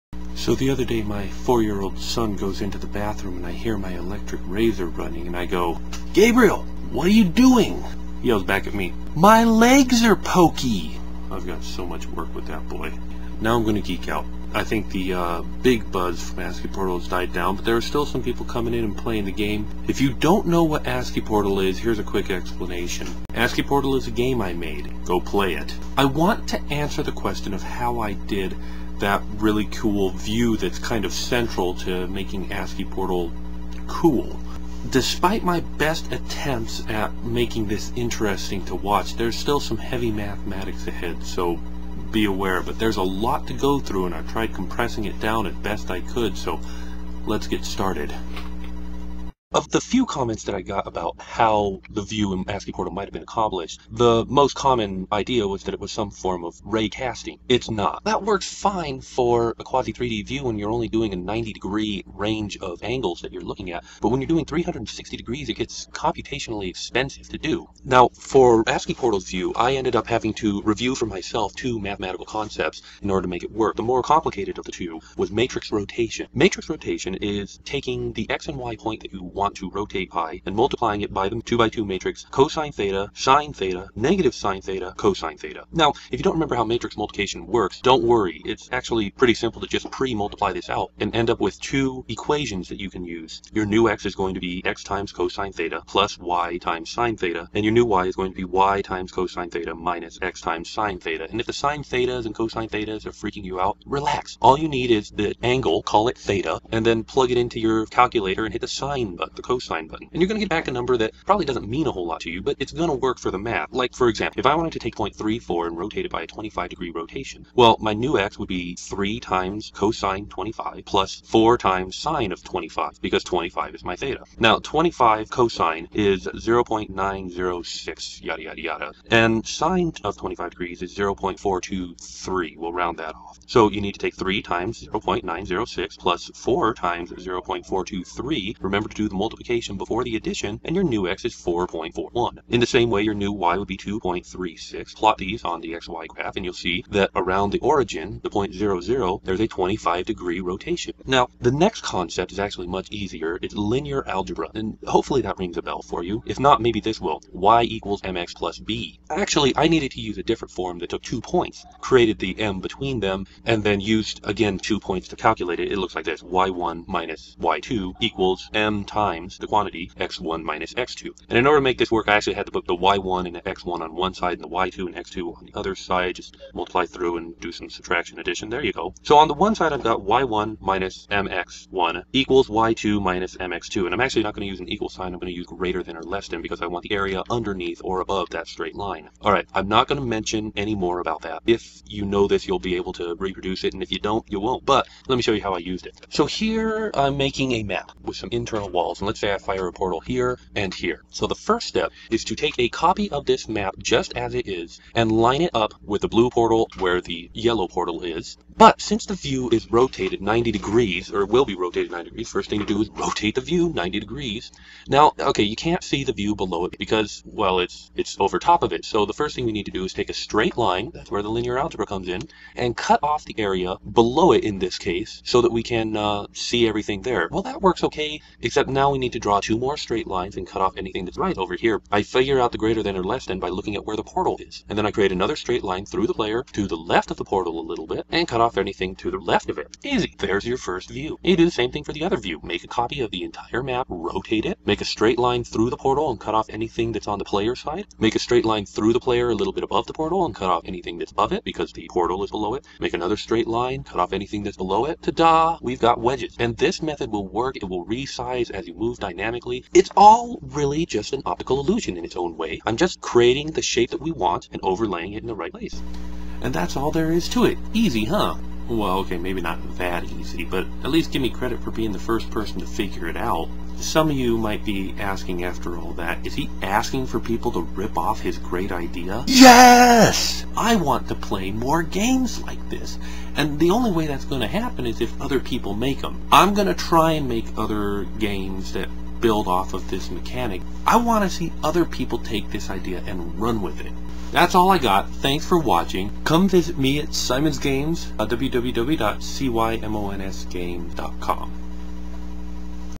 ASCIIpOrtal views how to
If you've been wondering how I did that cool view thing in ASCIIpOrtal, well here's the really rapid explanation.

Bear with me, guys, this is still a learning experience. The frame rate is up, but so is the grainy-ness. And that explanation went by way fast, but trust me, this video was long enough as is.

Tell me what you thought of the really sped up drawing illustration with the kinda sped up voice thing. Did it work? Did it not? Did it go to fast to be any good?

Hope you like the doodle, too.